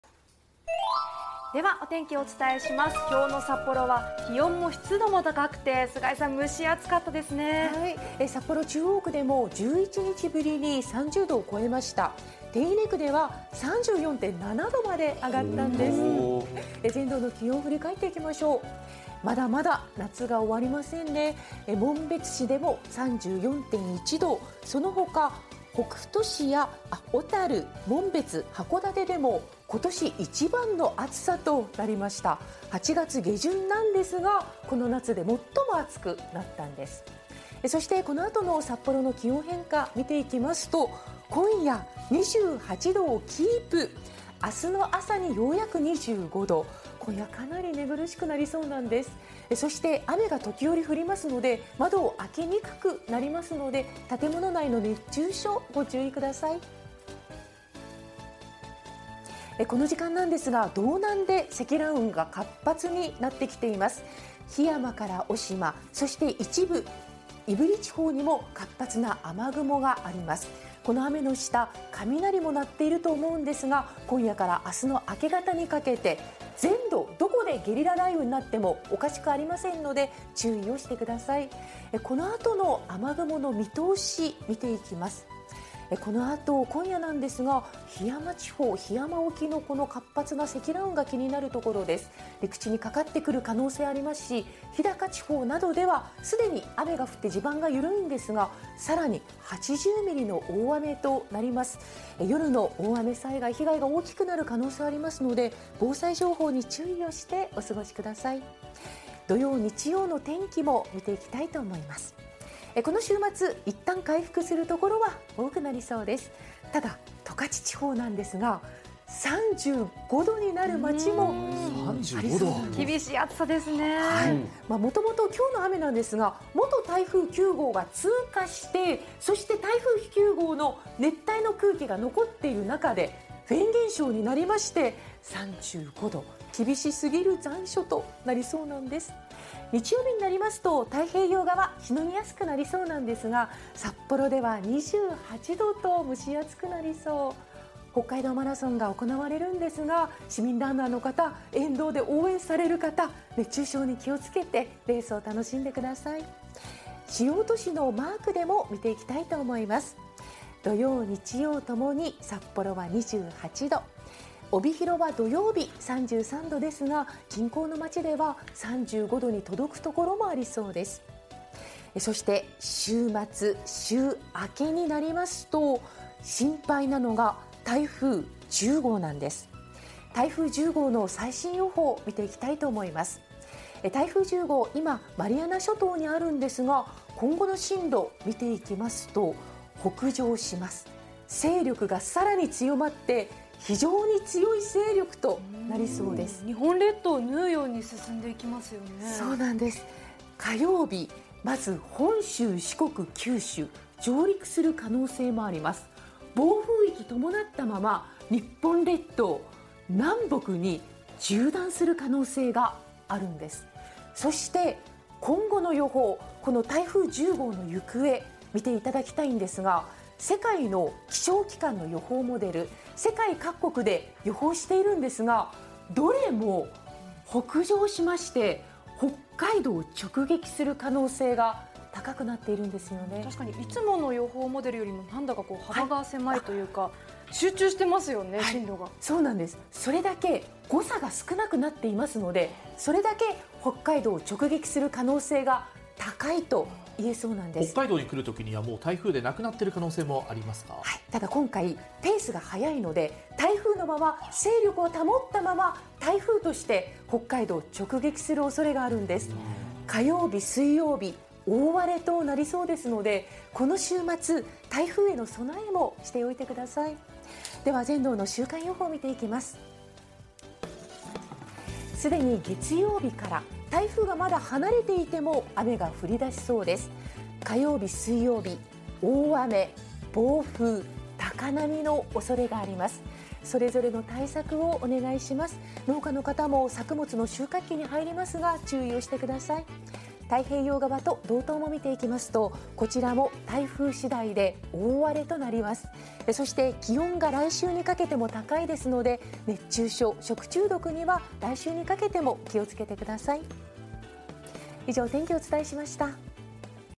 北海道【菅井さんの天気予報 8/23(金)】今夜からあす明け方にかけて“ゲリラ雷雨“”に要注意 週末は猛暑が復活！十勝地方で35℃か
（菅井貴子気象予報士が解説）午後5時30分現在

◆きょう23日(金)の気温
　元台風9号が、北海道を通過中です。
　熱帯の蒸し暑い空気を運び込み、
　日差しが少なく雨が降った所でも気温が上がりました。
　札幌市手稲区で34.7℃まで上がったほか、
　小樽、紋別、函館は、今年一番の高温となりました。
　今夜も気温が下がらず、25℃以上の所が多いでしょう。
　かなり寝苦しくなりますので、お休み前に適度な水分補給をして、
　夜間の熱中症対策もして下さい。

◆雨雲の予想
　きょう23日(金)午後4時35分現在、
　桧山地方から胆振地方にかけて、積乱雲が発達しています。
　激しい雷雨、ひょう、竜巻などに注意が必要です。
　登別市では雨量がすでに100ミリを超えていますが、
　あすにかけても、多い所で80ミリです。
　夜間の大雨災害は、被害が大きくなりやすいので、
　防災情報に気を付けてお過ごしください。

◆土日の天気
　・24日(土)
　晴れ間が戻る所が多くなりますが、
　道北を中心に、にわか雨の所があるでしょう。
　十勝地方はフェーン現象が加わり、
　猛暑日で、35℃以上の所があるかもしれません。
　・25日(日)
　宗谷地方で晴れるほかは、全道的に雲が多いでしょう。
　札幌では北海道マラソンが行われますが、
　最高気温は28℃で、湿度も高いでしょう。
　市民ランナーの方や沿道で応援される方も、熱中症に気をつけて下さい。

◆台風10号
　台風は、発達を続けながら週明けには“非常に強い”勢力となりそうです。
　27日(火)は、本州、四国、九州に上陸しそうです。
　その後、さらに北上を続ける見込みで、世界各国の予測を見ても
　北海道に直撃する可能性があります。
　来週半ばは、大荒れになりそうですので、
　大雨、強風、高波の対策は、早めにお願いします。

◆全道の週間予報　24日(土)～30日(金)
　週明けは、台風10号から遠くても、前線の影響で、全道的に雨が降るでしょう。
　27日(火)～28日(水)は、北海道も大荒れの天気となる可能性があります。
　また、来週にかけても、台風が運んだ熱帯の空気で、蒸し暑さが続きそうです。